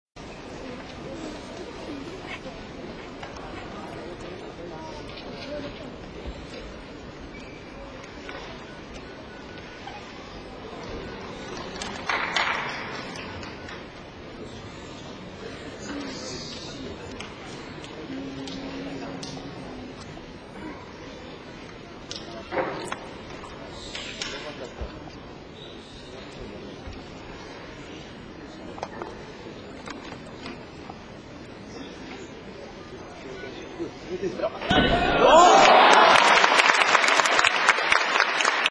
Yoshihiro Saito HB 2001 Japanese Nationals EF
1st in the High Bar Final with the day's best score of 9.725 in all events. What a shame JPN team missed the 2001 World Championships. The gymnast could have got what he deserved!